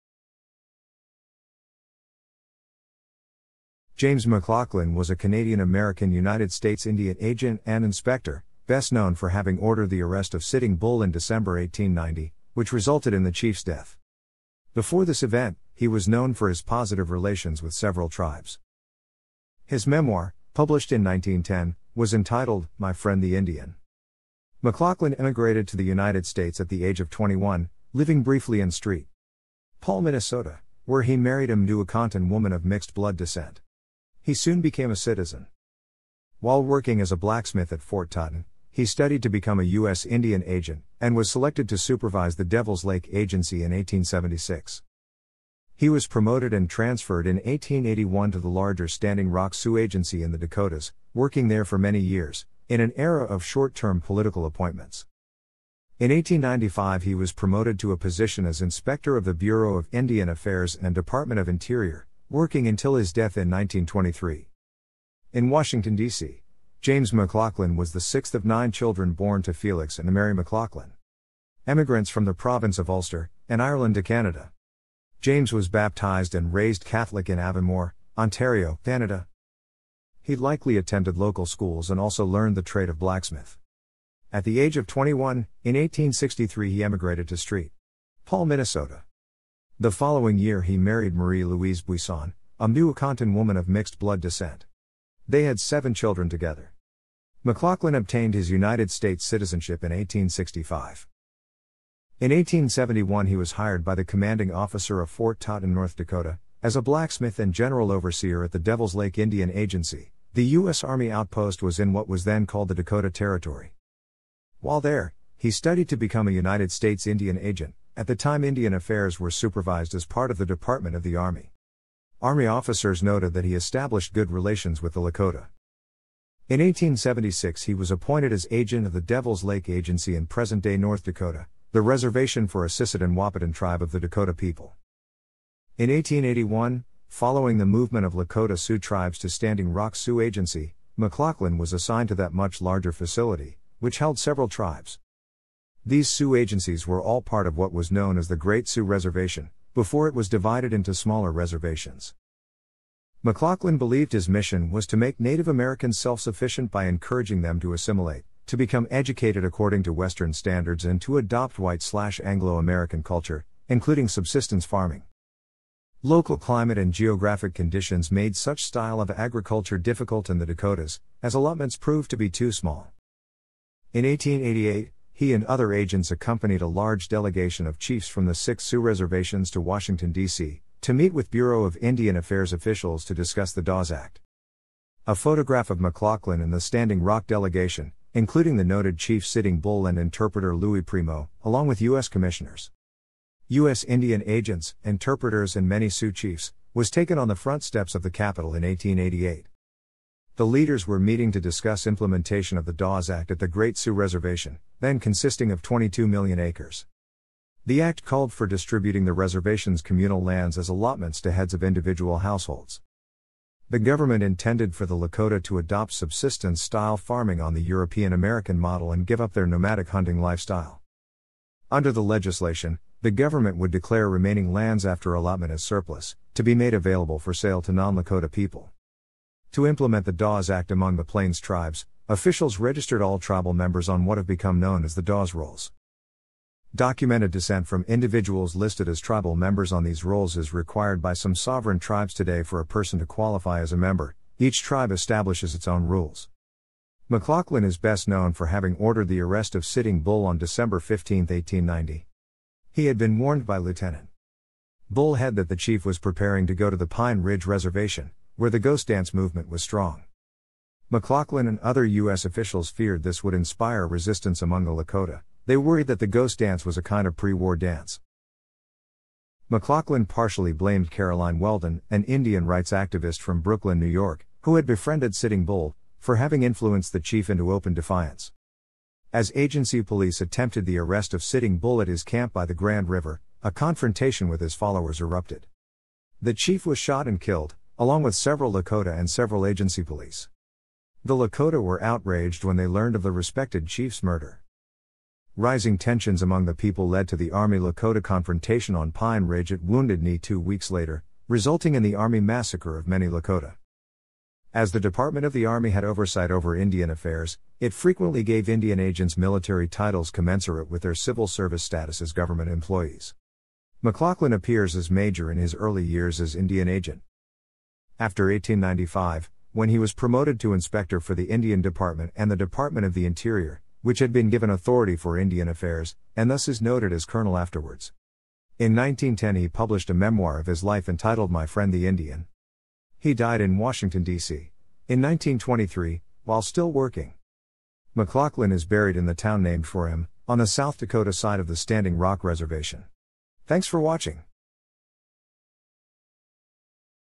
James McLaughlin (Indian agent)
Pre-Confederation_Canadian_emigrants_to_the_United_States
People_from_the_United_Counties_of_Stormont,_Dundas_and_Glengarry
James McLaughlin （February 12, 1842 – July 28, 1923） was a Canadian-American United States Indian agent and inspector, best known for having ordered the arrest of Sitting Bull in December 1890, which resulted in the chief's death.
Before this event, he was known for his positive relations with several tribes.
His memoir, published in 1910, was entitled, My Friend the Indian.
McLaughlin emigrated to the United States at the age of 21, living briefly in St.
Paul, Minnesota, where he married a Mdewakanton woman of mixed-blood descent.
He soon became a citizen.
While working as a blacksmith at Fort Totten, he studied to become a U.S. Indian agent, and was selected to supervise the Devils Lake Agency in 1876.
He was promoted and transferred in 1881 to the larger Standing Rock Sioux Agency in the Dakotas, working there for many years, in an era of short-term political appointments.
In 1895 he was promoted to a position as Inspector of the Bureau of Indian Affairs and Department of Interior, working until his death in 1923
in Washington, D.C. James McLaughlin was the sixth of nine children born to Felix （Catholic） and Mary （Prince, Protestant） McLaughlin,
emigrants from the Province of Ulster, in Ireland to Canada.
James was baptized and raised Catholic in Avonmore, Ontario, Canada.
He likely attended local schools and also learned the trade of blacksmith.
At the age of 21, in 1863 he emigrated to St.
Paul, Minnesota.
The following year he married Marie Louise Buisson, a Mdewakanton woman of mixed-blood （Québécois and Scottish） descent.
They had seven children together.
McLaughlin obtained his United States citiz...